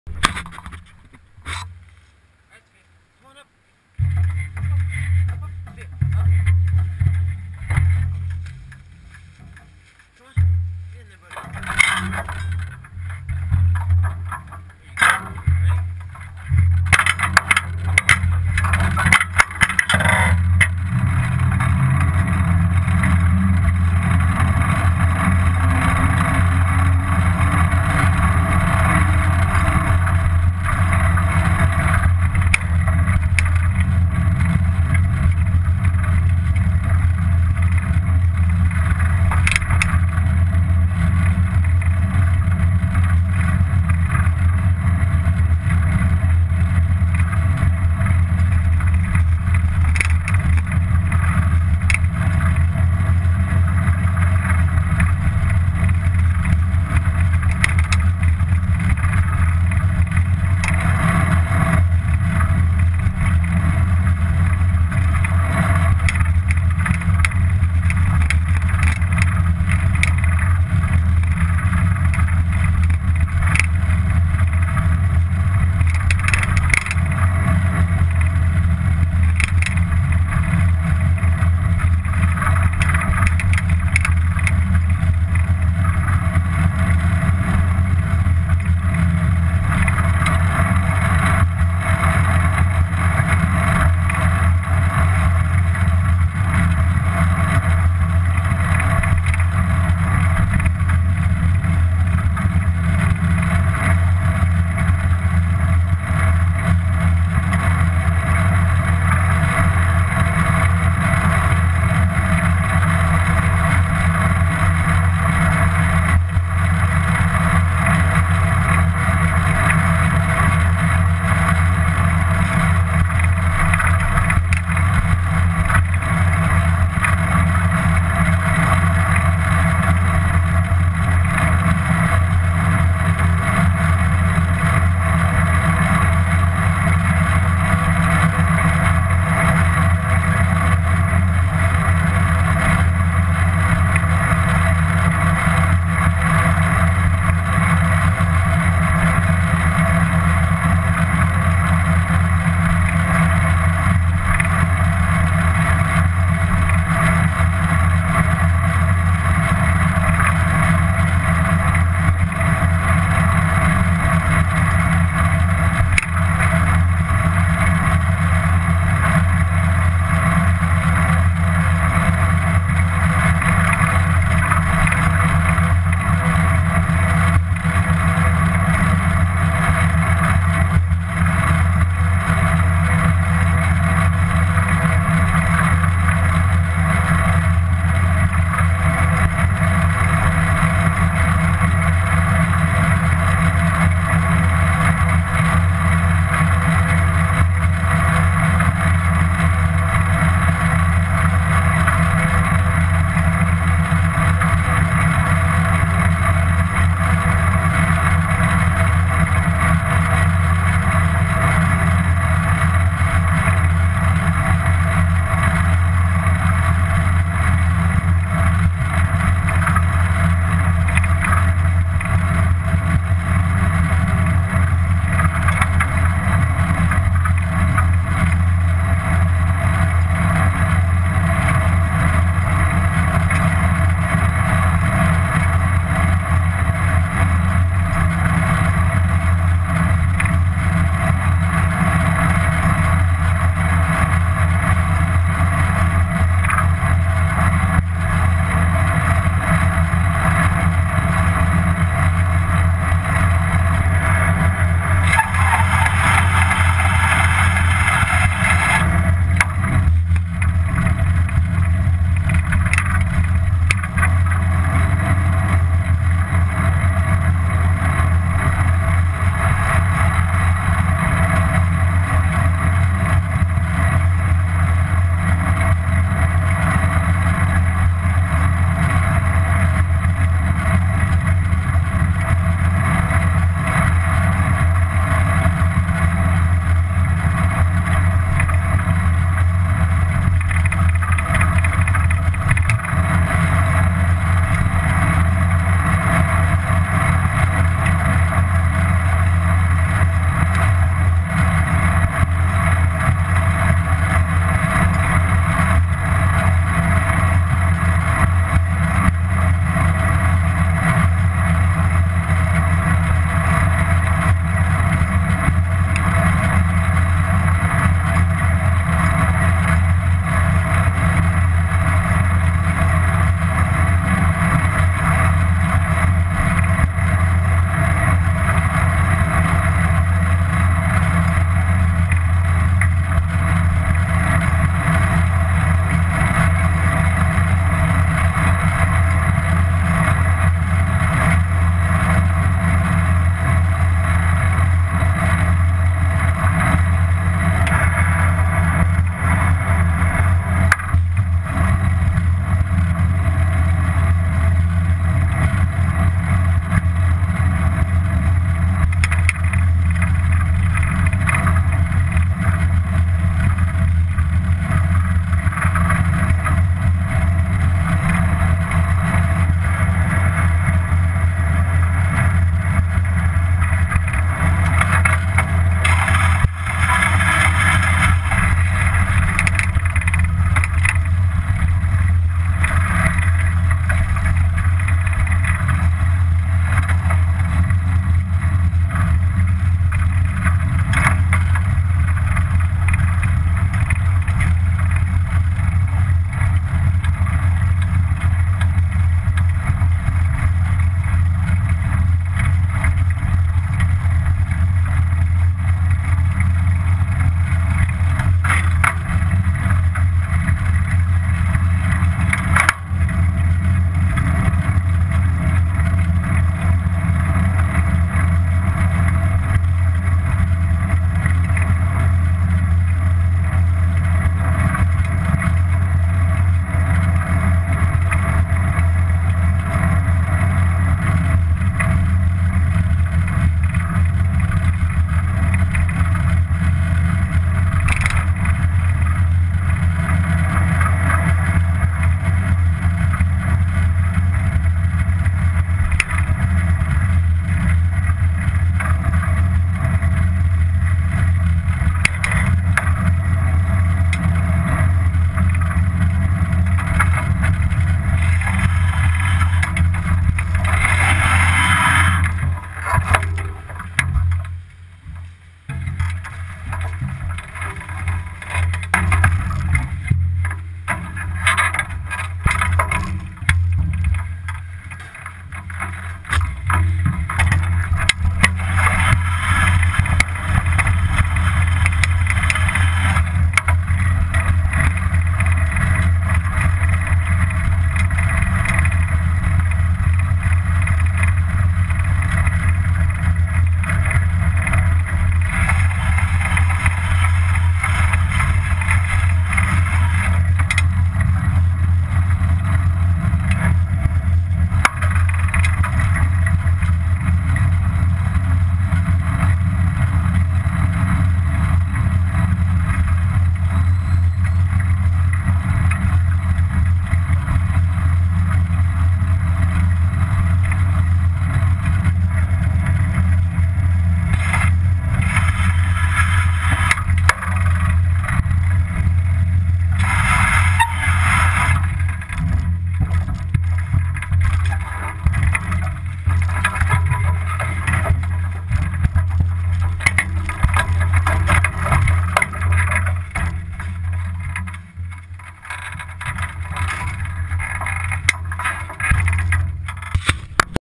My commute to work
Every morning we ride to work, me by bike and she by trailer. This is a short movie of our commute